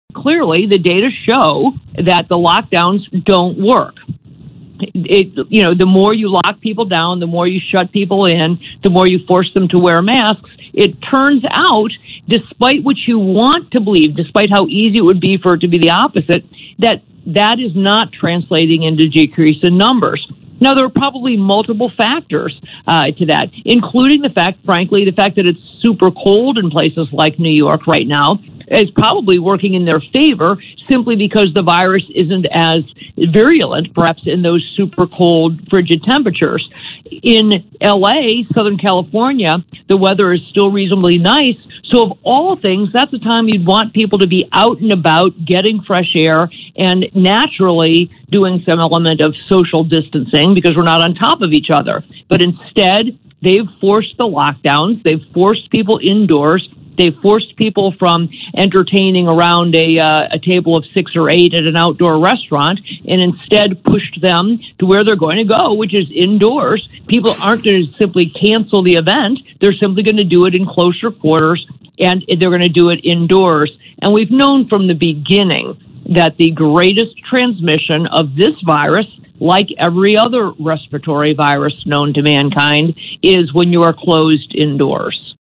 Why COVID-19 lockdowns don't work
States like California keep doubling down on failure.
November 14 - Closed indoor dining in 41 counties
November 20 - Statewide curfews
December 6 - Closed outdoor dining